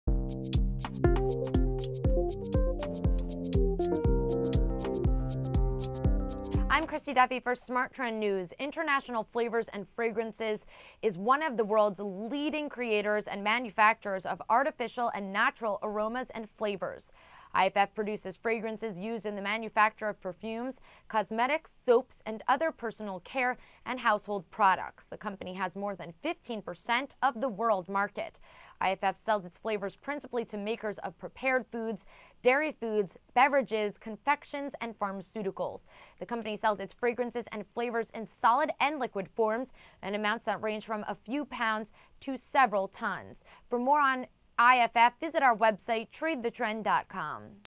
Company Profile: Intl Flavors & Fragrances (NYSE:IFF)
International Flavors & Fragrances (IFF) is one of the world's leading creators and manufacturers of artificial and natural aromas and flavors, IFF produces fragrances used in the manufacture of perfumes, cosmetics, soaps, and other personal care and household products. The company has more than 15% of the world market. IFF sells its flavors principally to makers of prepared foods, dairy foods, beverages, confections, and pharmaceuticals. The company sells its fragrances and flavors in solid and liquid forms in amounts that range from a few pounds to several tons